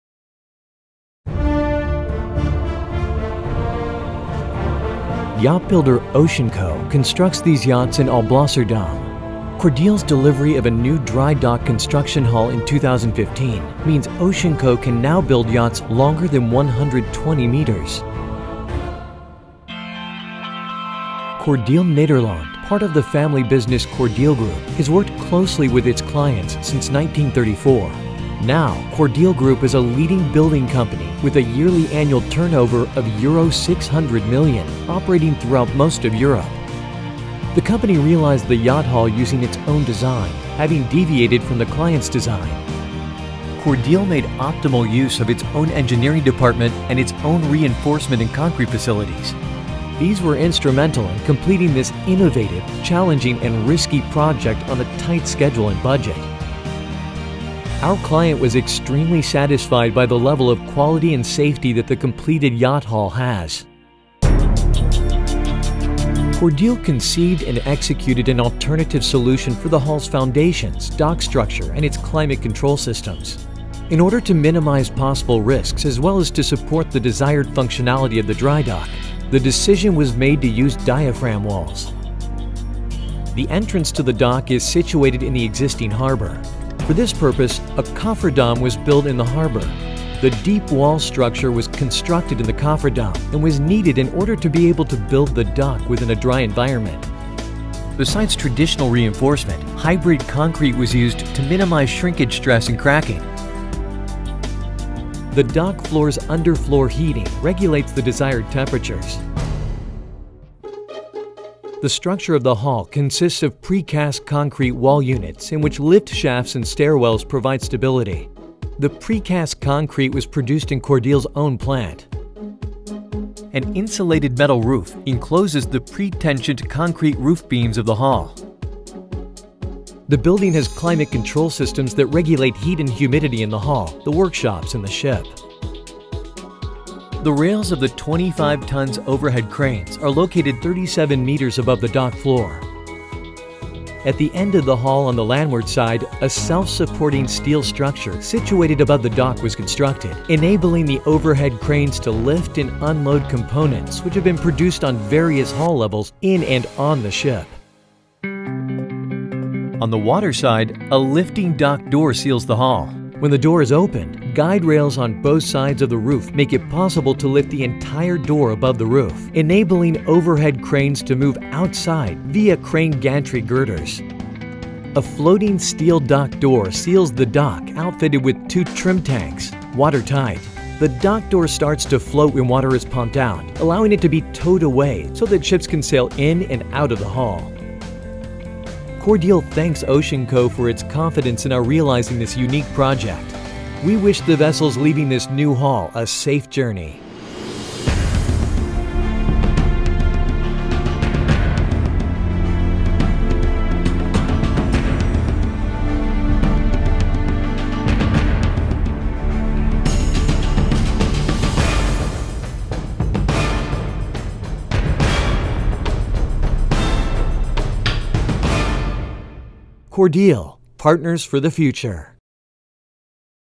Cordeel nieuwbouw jachtbouwhal Oceanco
Jachtbouwer Oceanco bouwt jachten in Alblasserdam. Om jachten langer dan 120 meter te kunnen bouwen leverde Cordeel in 2015 voor hen deze nieuwe jachtbouwhal op.